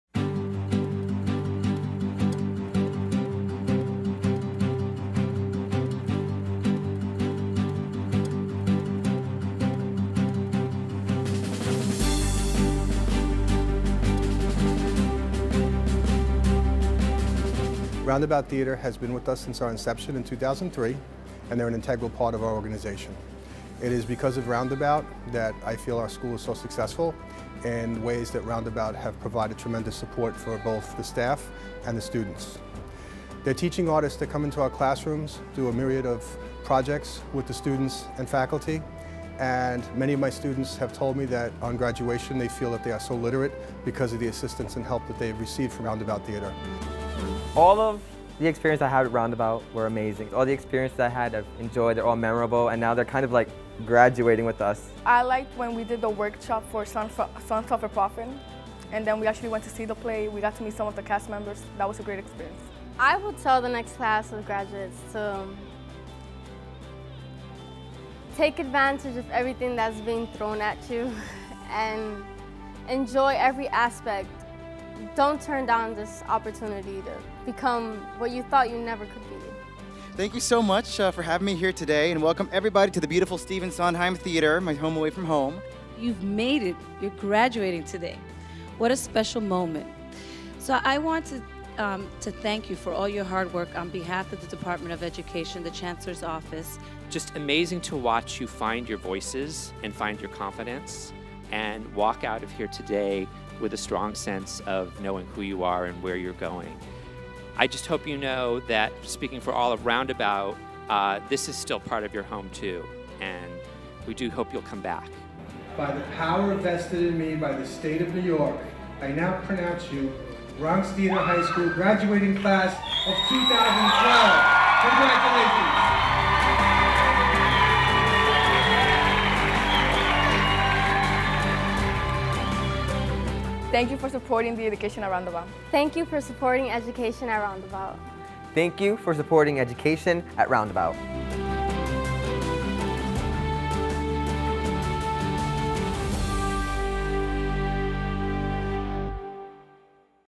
Bronx Theatre High School Graduation
On Tuesday, Bronx Theatre High School's Class of 2012 graduated on Broadway at the Stephen Sondheim Theatre.  This was no ordinary graduation as these students have been partnering with Education@Roundabout since their freshman year.  The senior class took to the Sondheim stage with confidence and a clear appreciation for theatre-based curriculum that's woven into all subject areas in Roundabout's partner schools.  There were solos, ensemble performances and even a keynote address from Raymond Lee (currently playing the role of John in ANYTHING GOES).